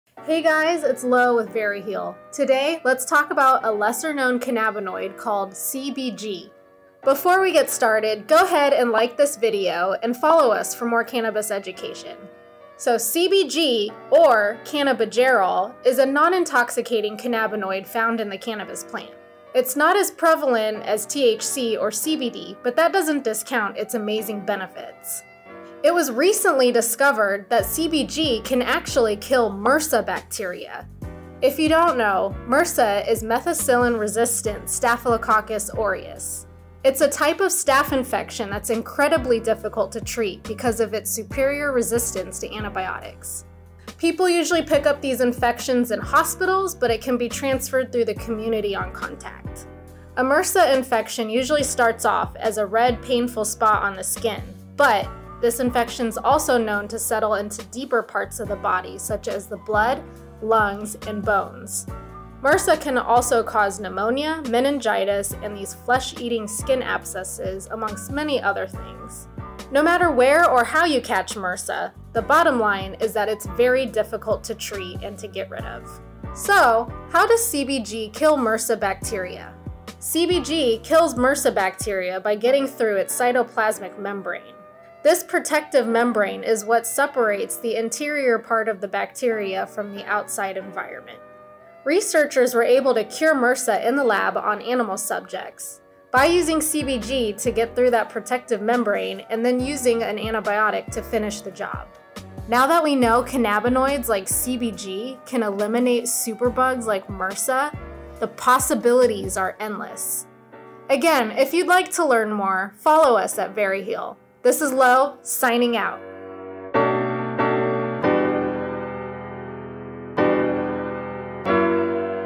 Can Cannabis Kill MRSA? Presenting... CBG!
Researchers were able to successfully eliminate MRSA bacteria using cannabinoids found in cannabis plants. This is a huge victory for cannabis research! MRSA infections are very hard to get rid of due to the bacteria's resistance to antibiotics.

If you have any questions at all don't hesitate to reach out, we are more than happy to assist you every step of the way.